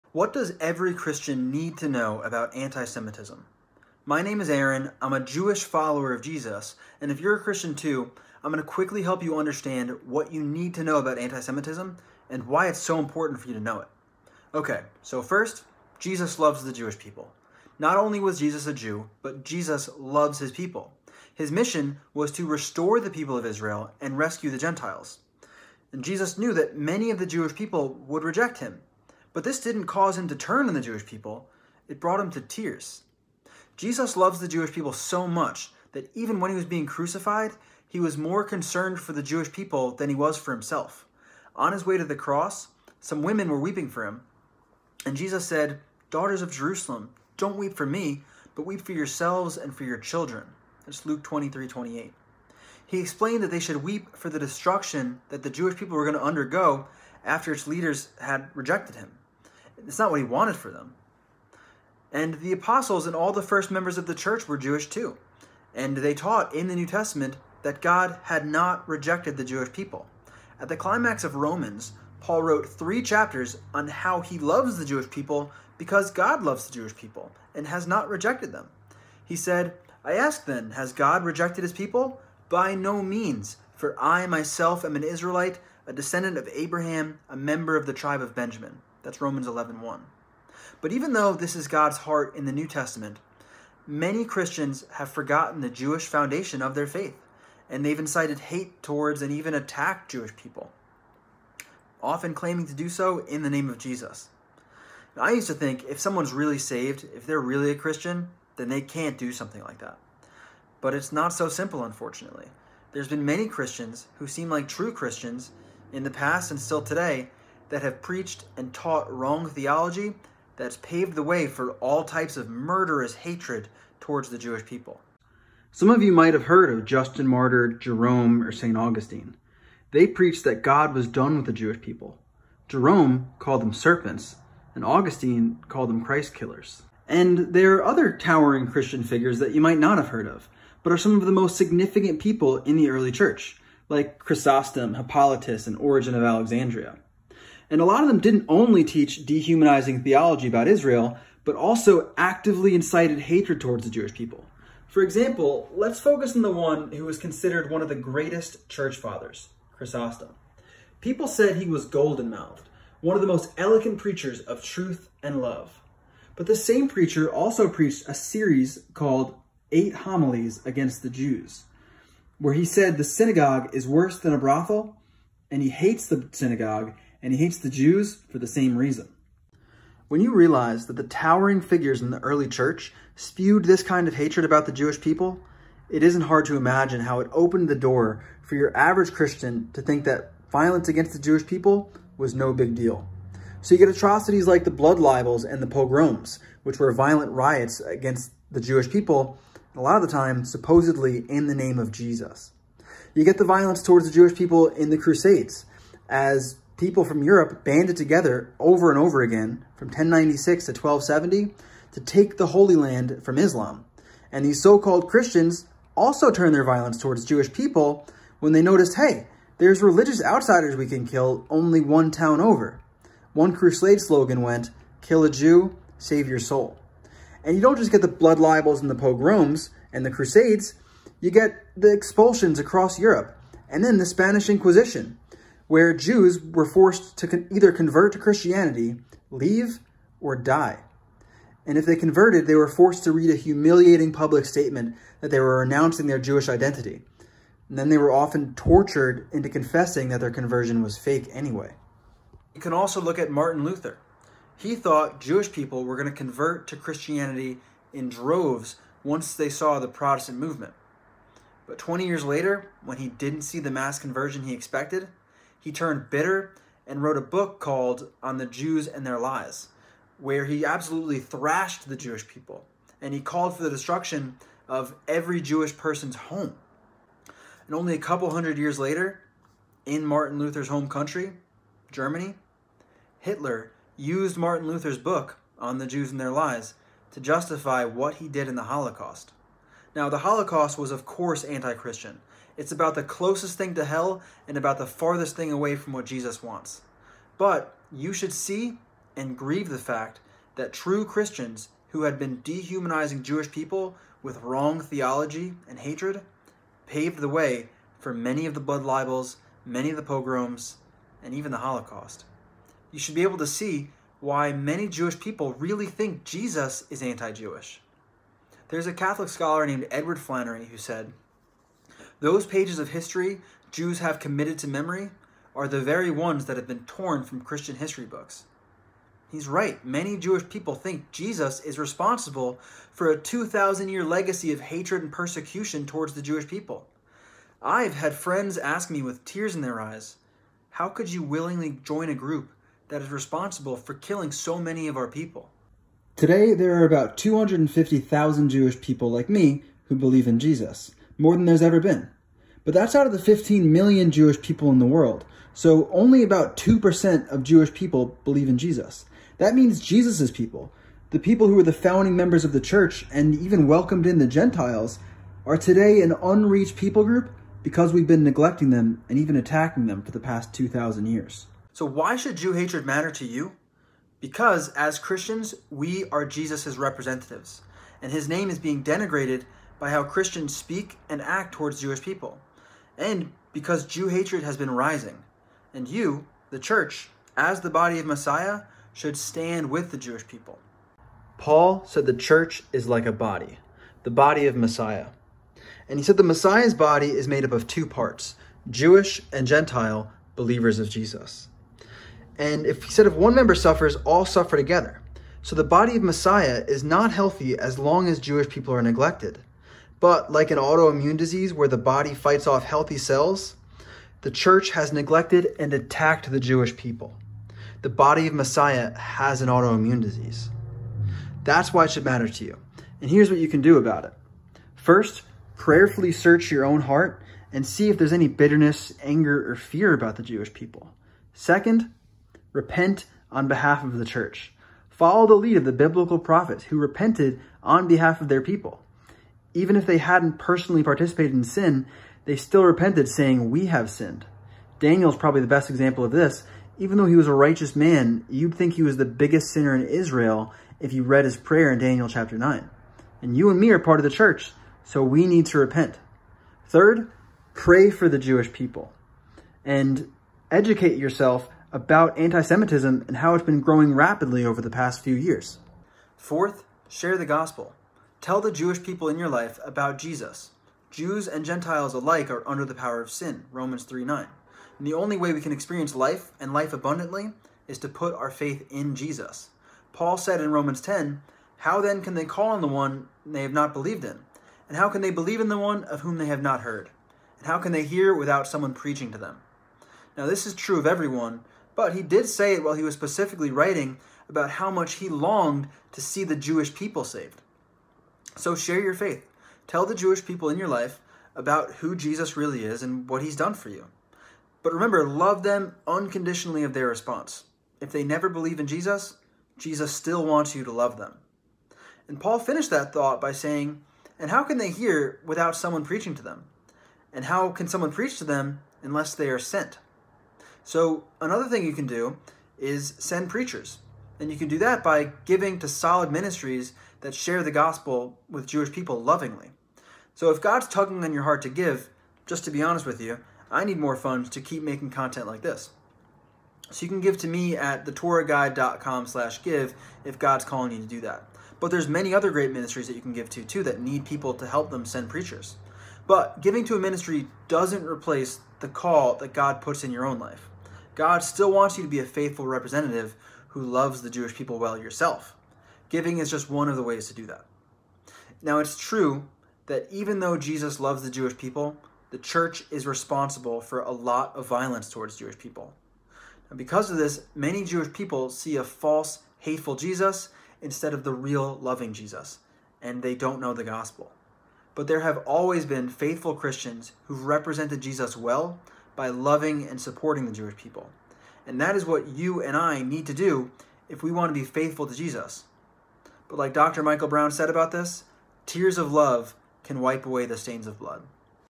What Every Christian Needs to Know about Antisemitism
This is a brief introduction to:
• Jesus’ heart for the Jewish people
• How the Church has treated the Jewish people
• What you should do about Antisemitism

To learn more start with:
• Our Hands are Stained With Blood - Dr Michael L. Brown
• Jesus Masked: Anti-Jewish Theologies - Dr Theresa Newell